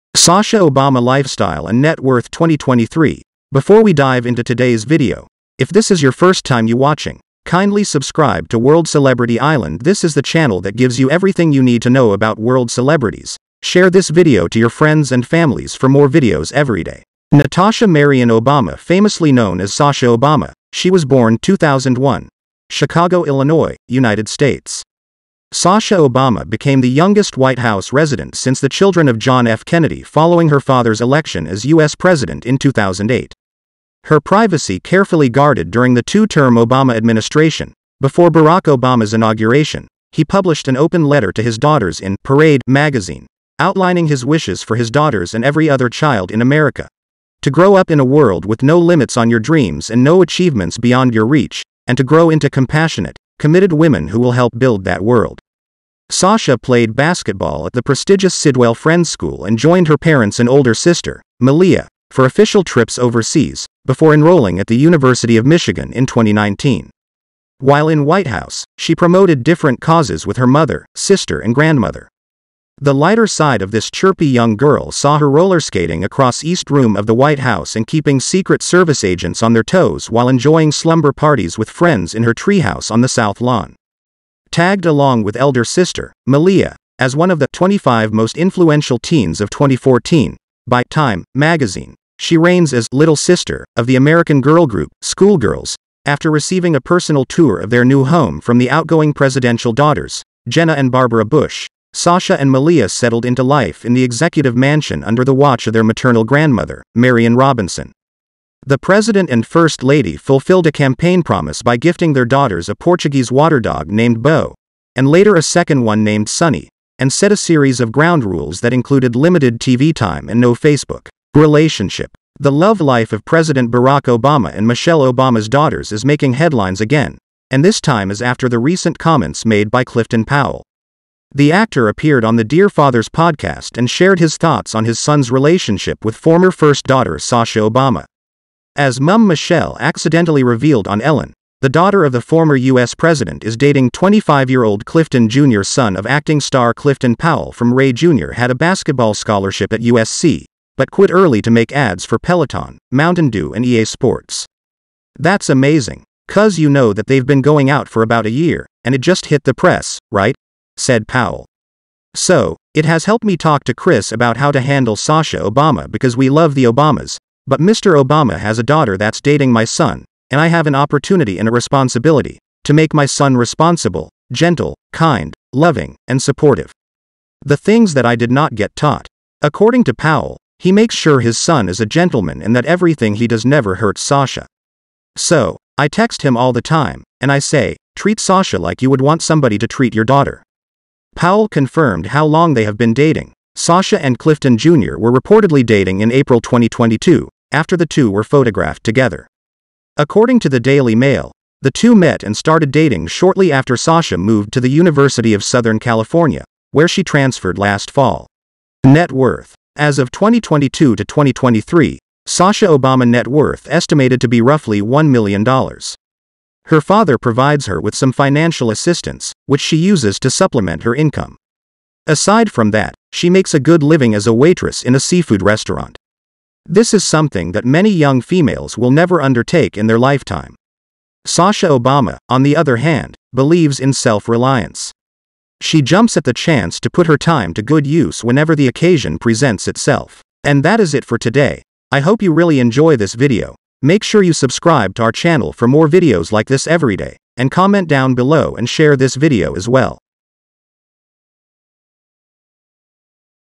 Sasha Obama CRAZY Lifestyle 2023 ★ Net worth! Income! House! Cars Boyfriend Family
Sasha Obama has an amazing liefstyel and net worth in this 2023 and  In this video we take a look inside the life of Sasha Obama.